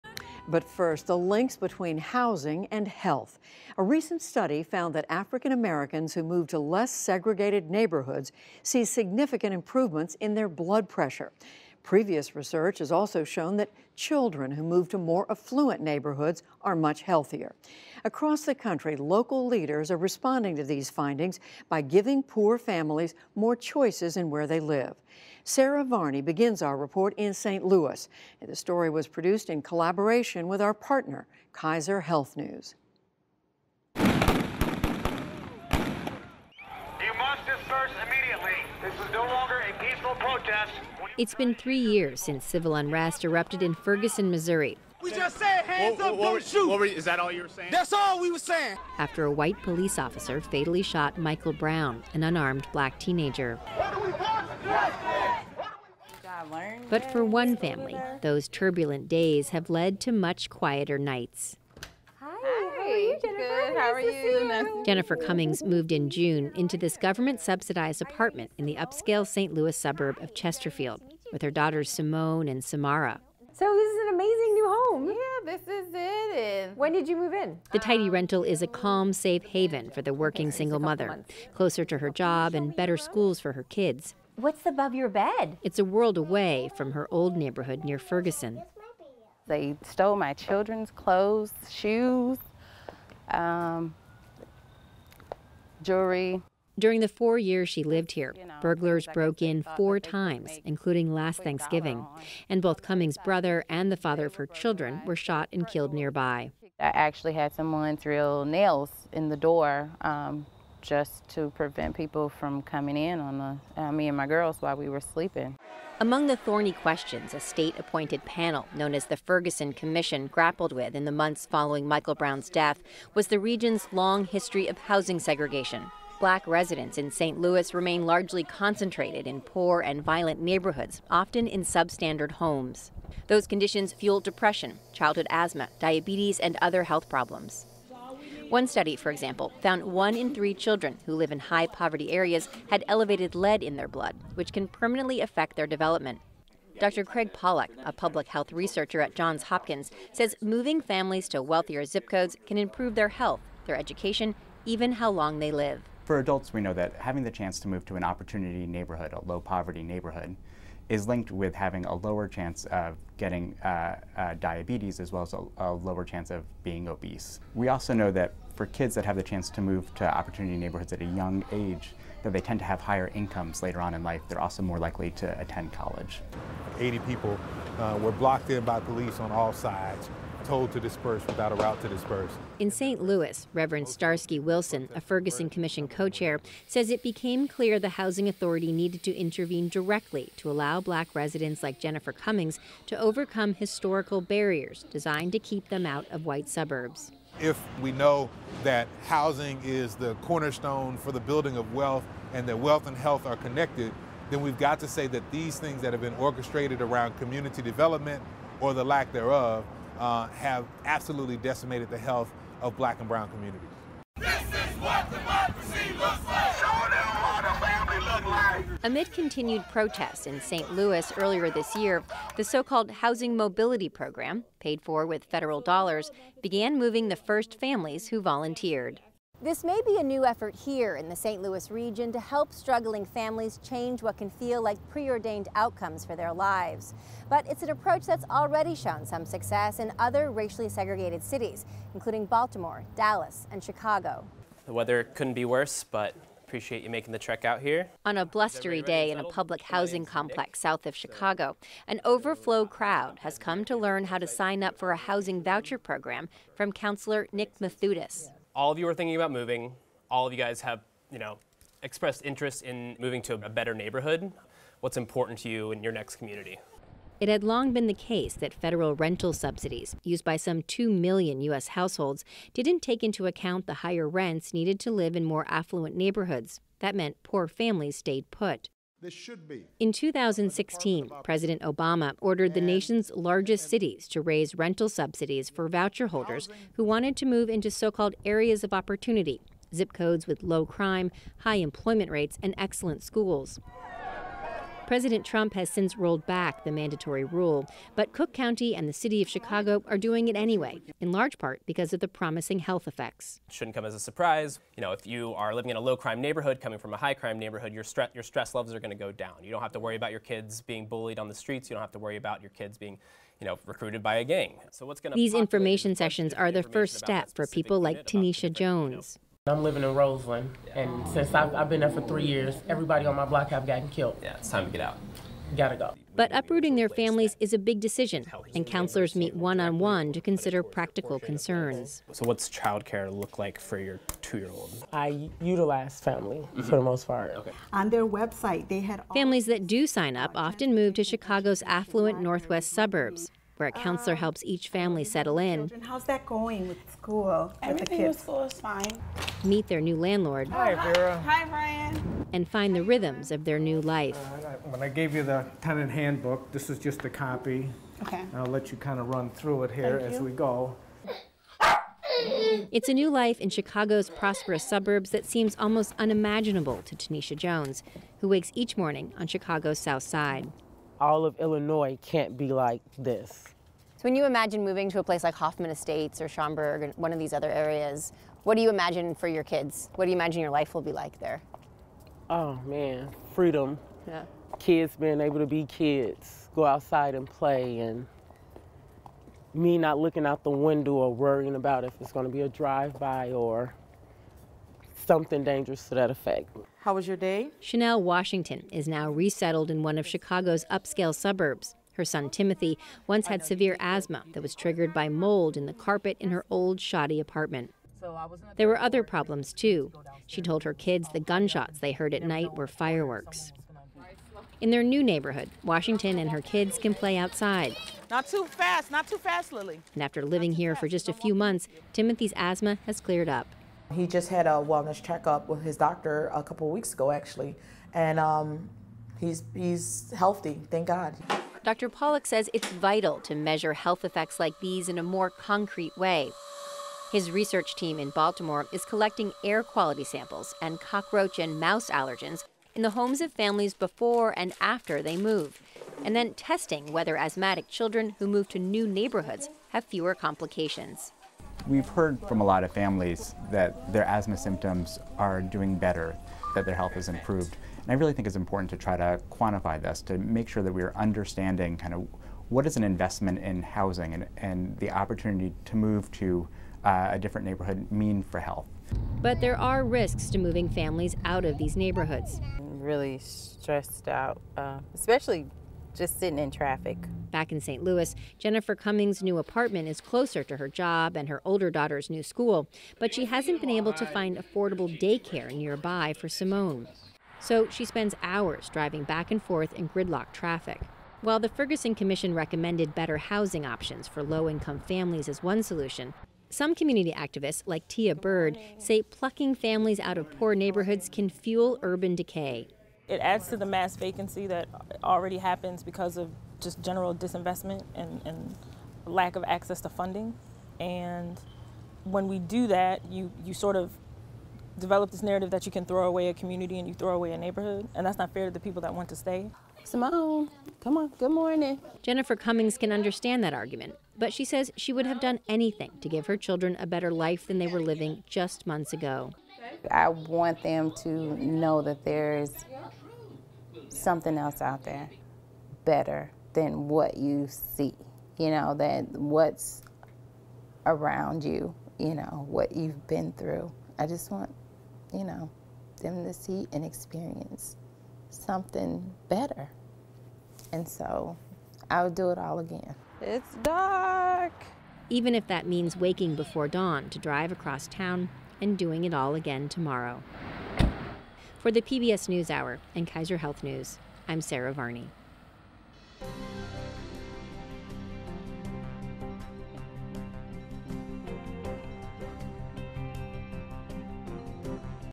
Giving poor families more choices in where to live can greatly improve health
When low-income Americans are concentrated in substandard homes in struggling or violent neighborhoods, it has tangible consequences for well-being. Research confirms that moving families into less segregated neighborhoods improves overall health, and some communities are giving families vouchers to relocate. Special correspondent Sarah Varney of Kaiser Health News reports.